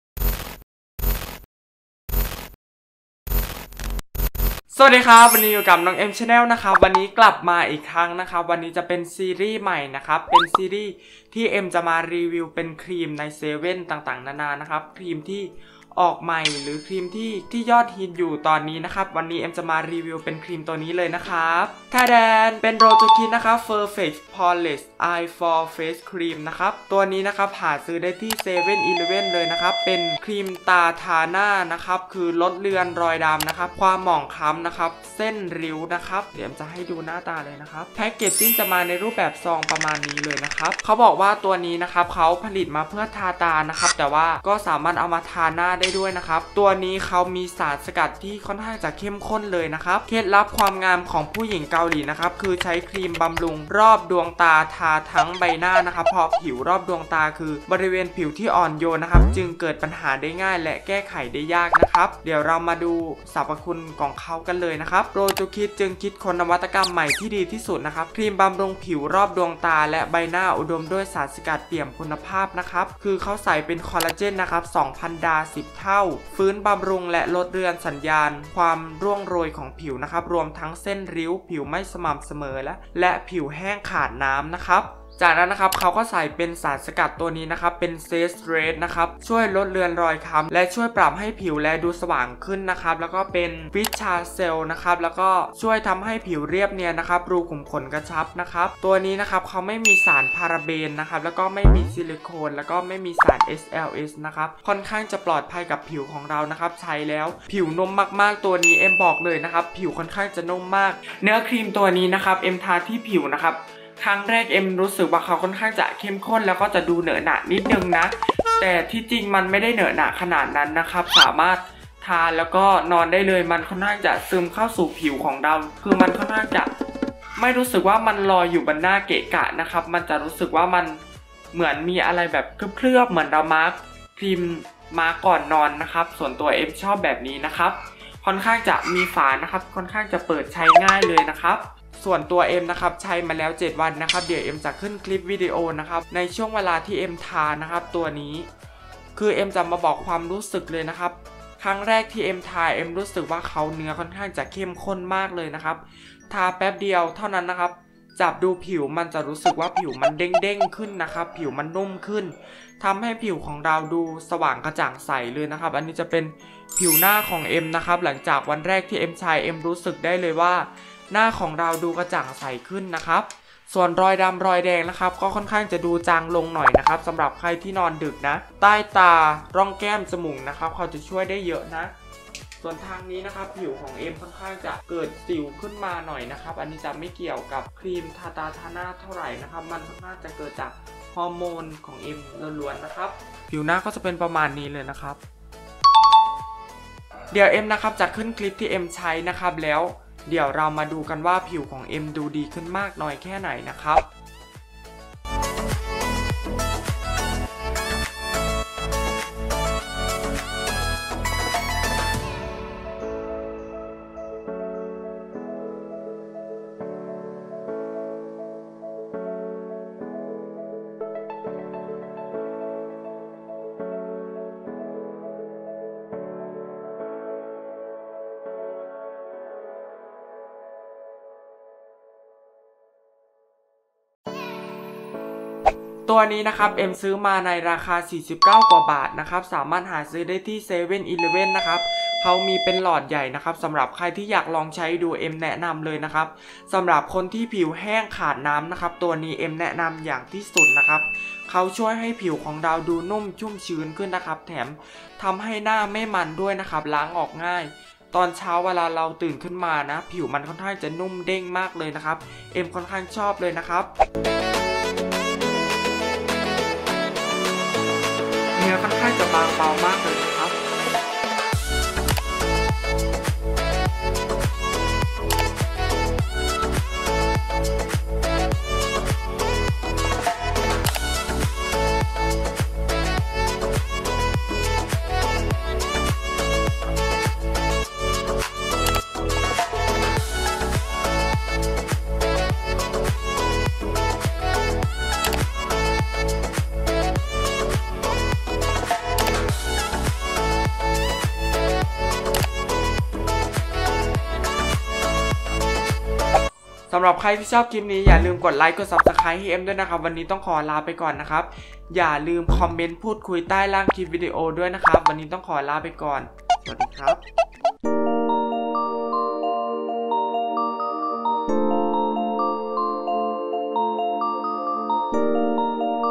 •รีวิวครีม 7-11• EP. 1 ครีมทาตาทาหน้า `Rojukiss ผิวดีงาม!!! คอลลาเจน 10 เท่า`| nongmchannel
Fanpage : nongm_channel

Camera : Sony a5100 เลนส์ kit